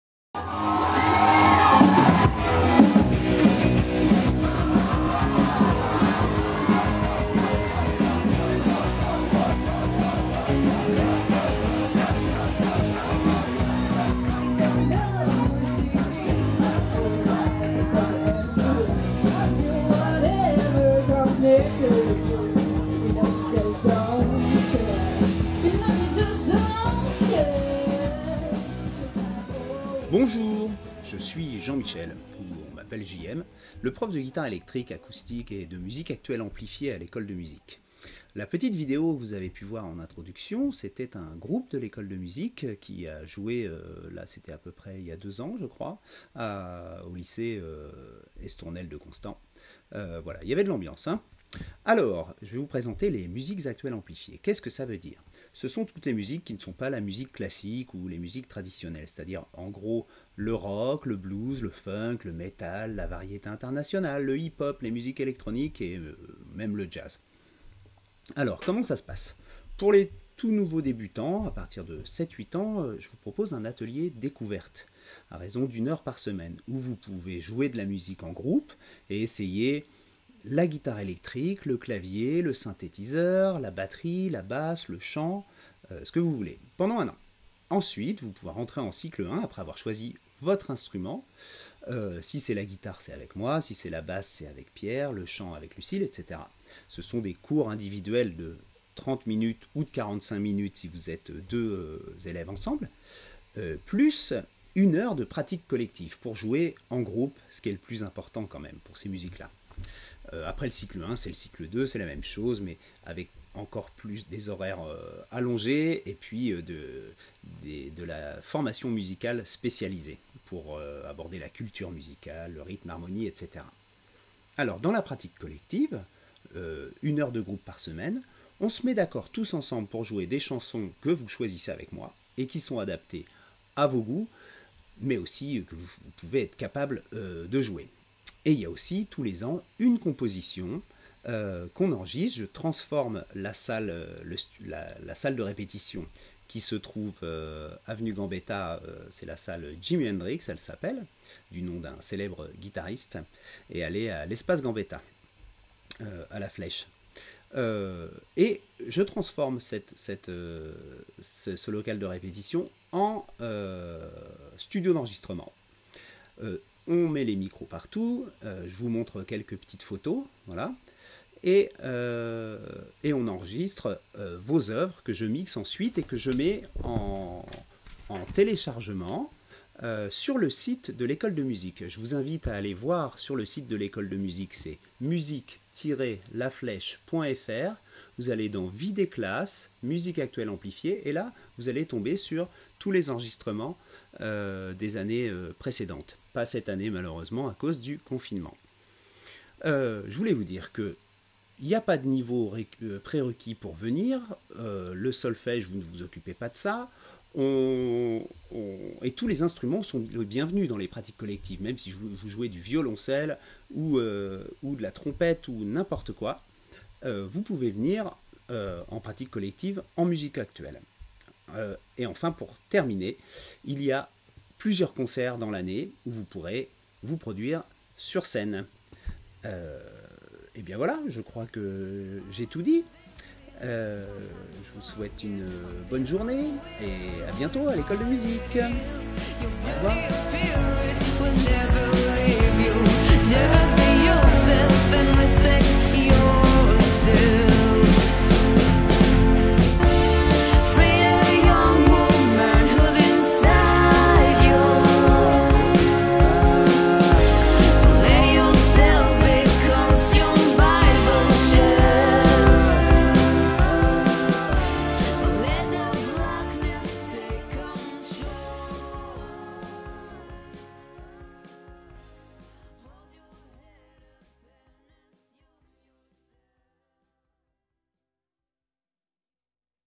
La guitare électrique (musiques actuelles)
Les professeurs de l'école municipale de musique de La Flèche présentent leur instrument.